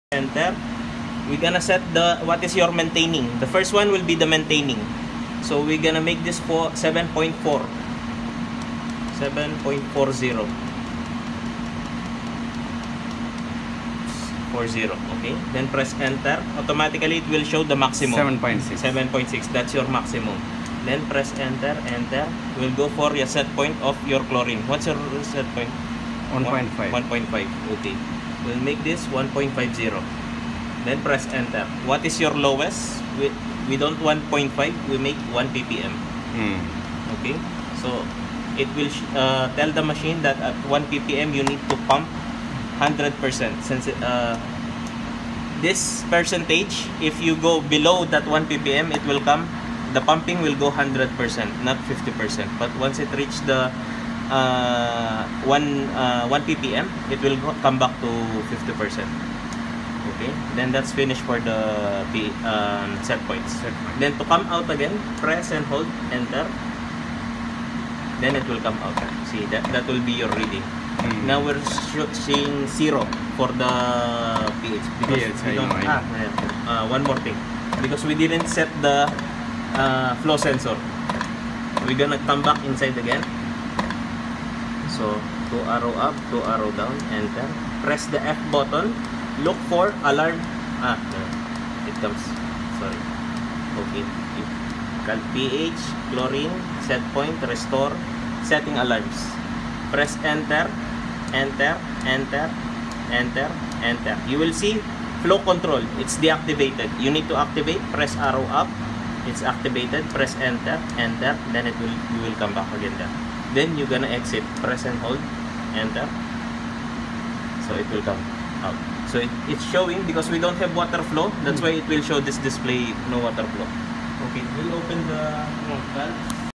Swimming pool Auto Chemical Pump Caliberation For Chlorine
Caliberation of Chlorine Auto Chomical Injection Pump For Swimming Pool, Water Features & Jacuzzis